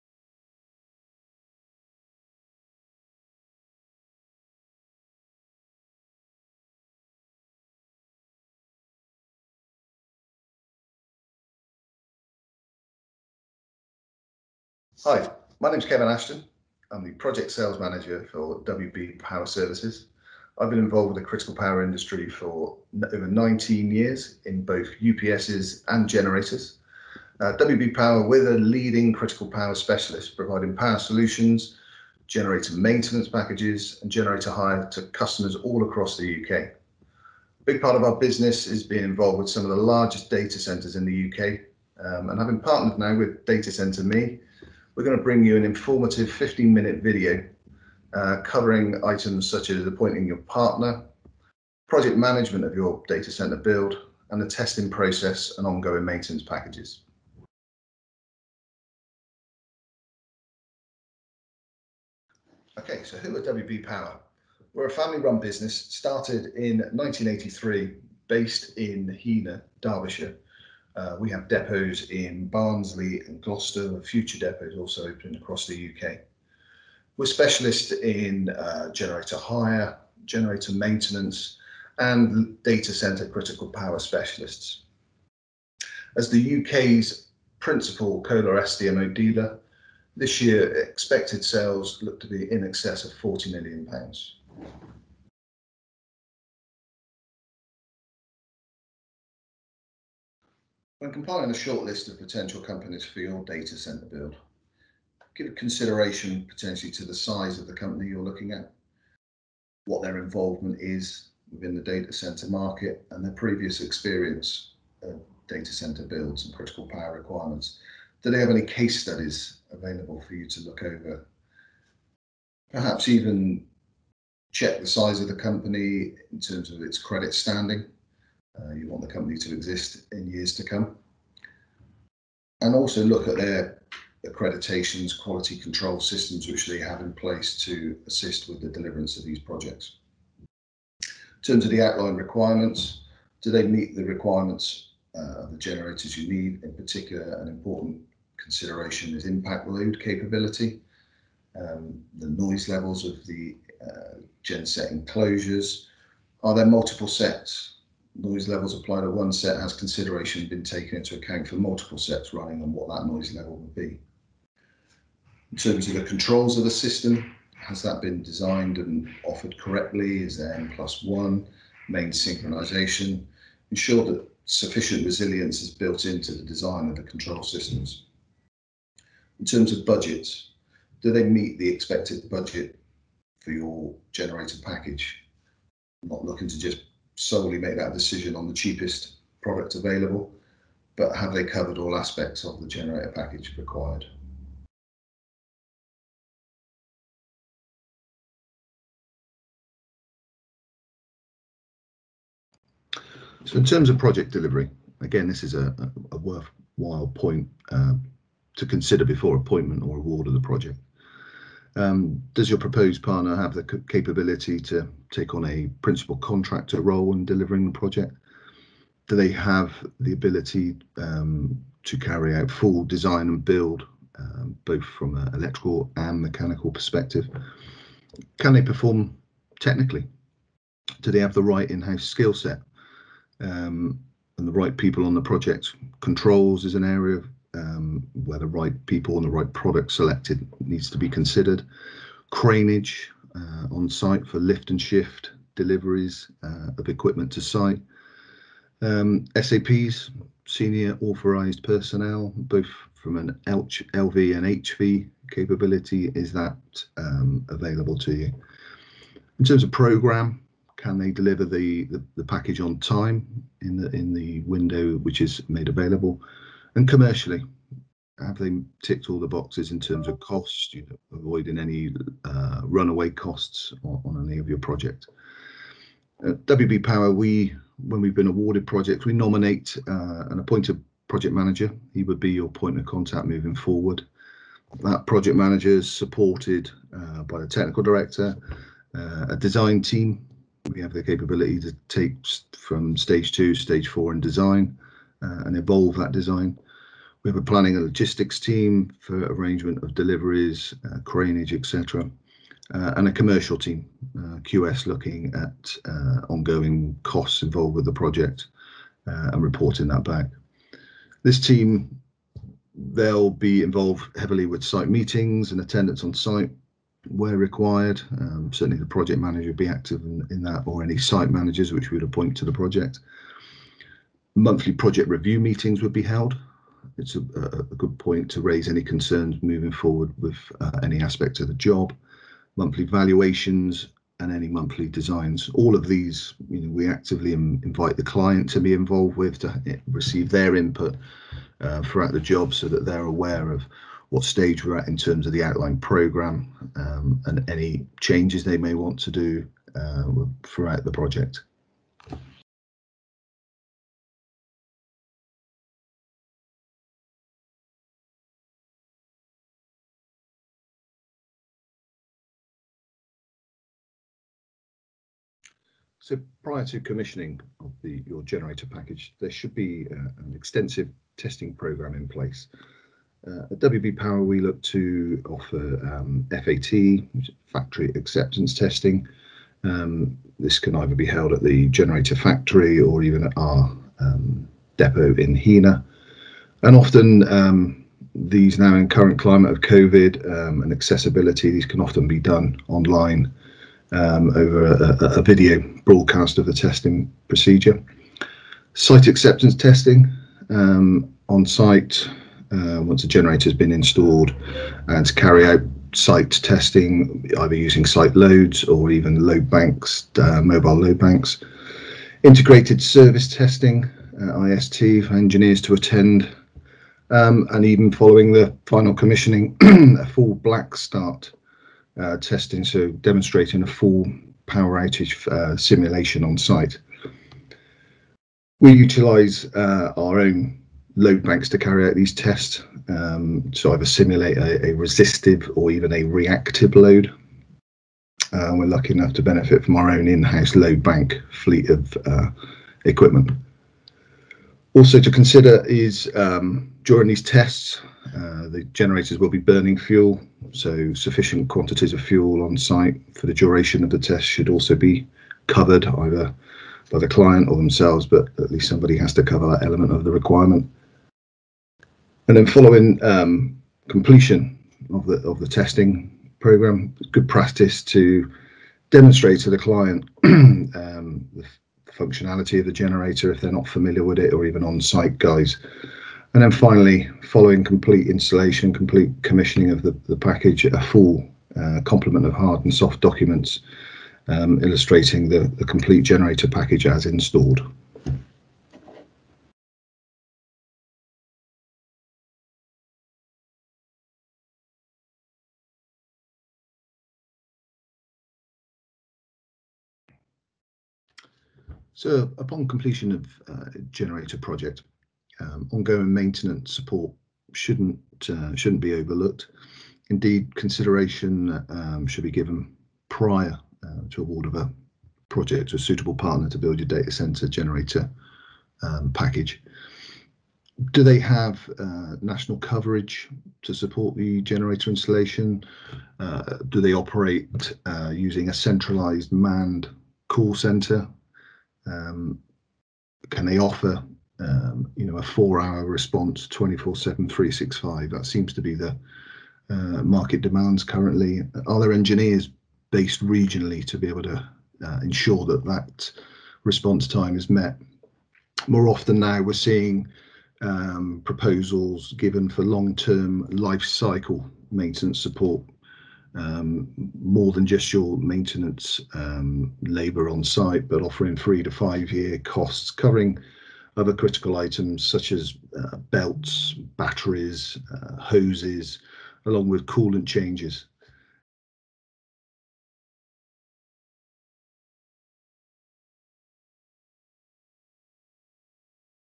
Session 9 Data Centre power solutions; How to be sure you select the right partner
Kevin Ashton, Project Sales Manager, WB Power Services

DCME GOES VIRTUAL 6th & 7th October 2020